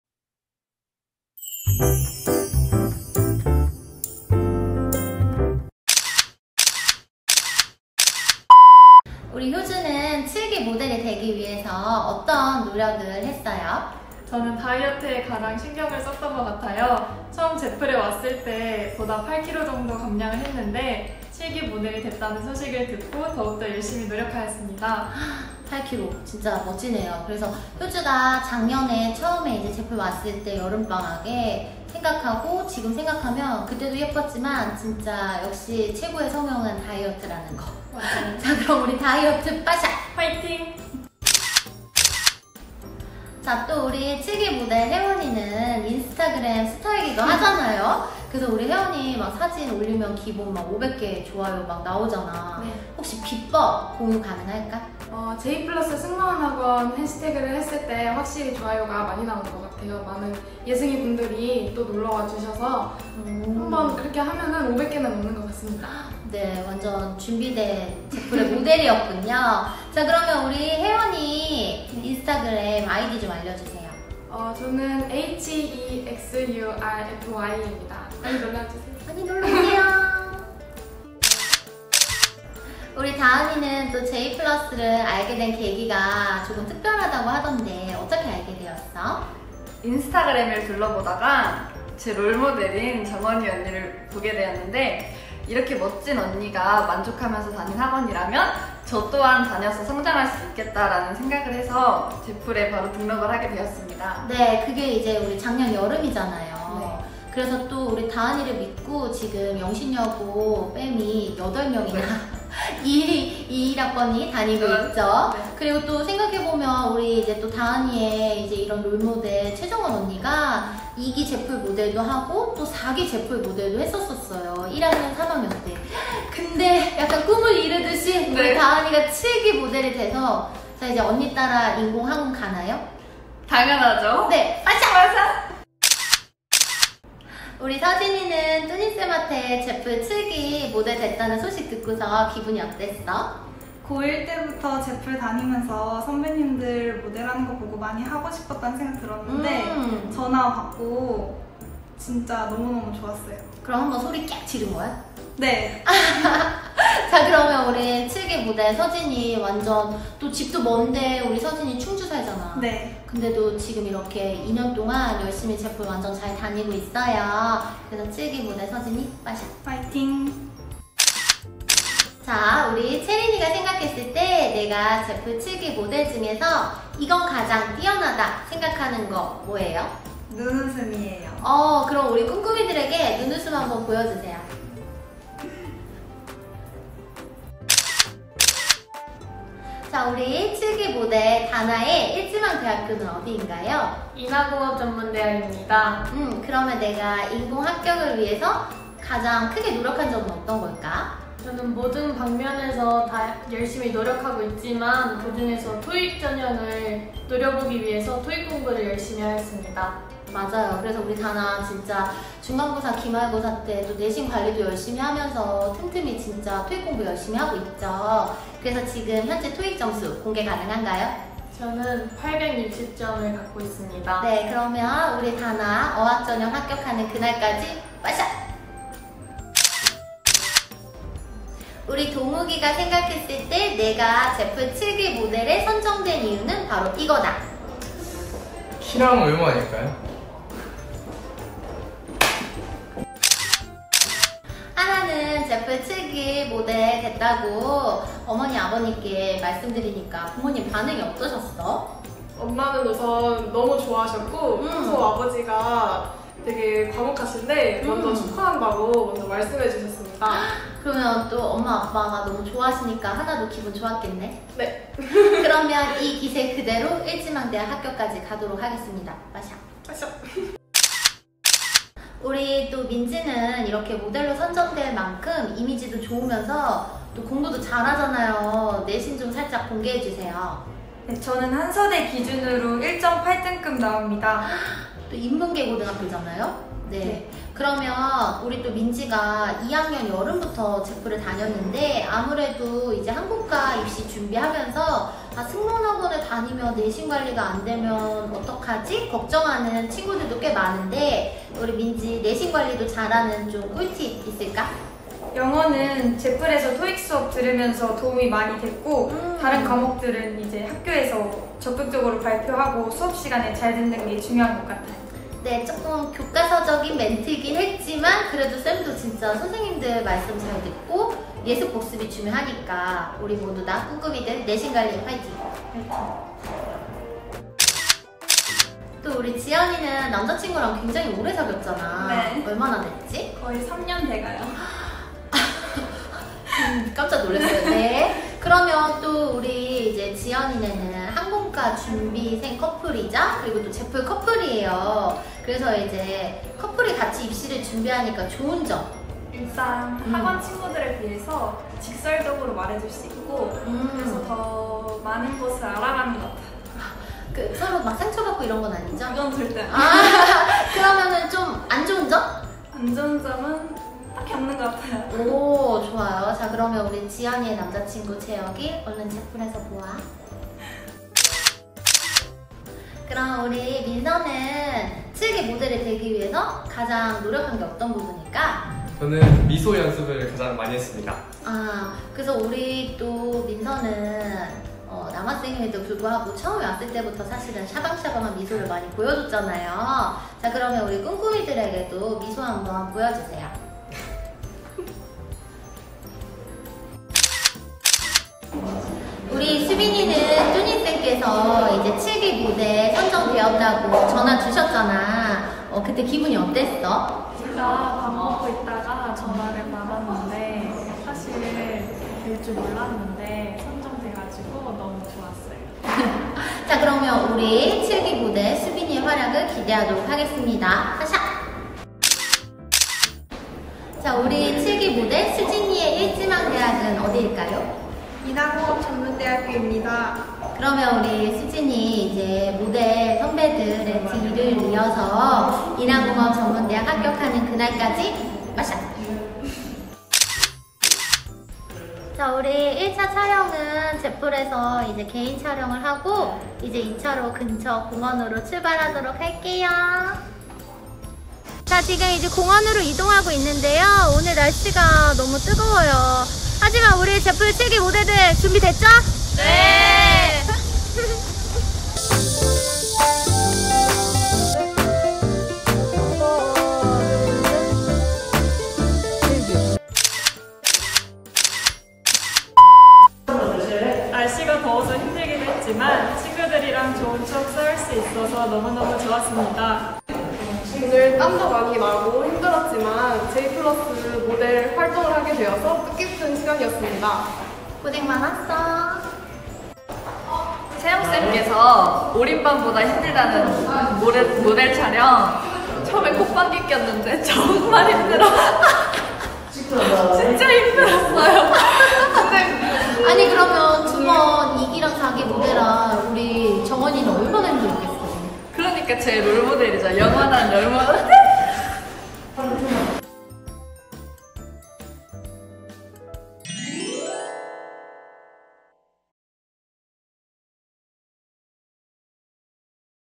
[꿈윙TV] 제이플러스 7기 모델 촬영 VLOG (항공과준비생, 예비승무원 일상)
안녕하세요. 꿈을 품고 윙을 달다 책의 저자 연희쌤이에요:)
오늘은 제이플러스 7기 모델 촬영 브이로그예요.
13명의 모델들의 활약 기대해 주세요.
앞으로 더 좋은 영상 많이 올릴게요~ 오늘도 화이팅 하세요! 합격캔두!!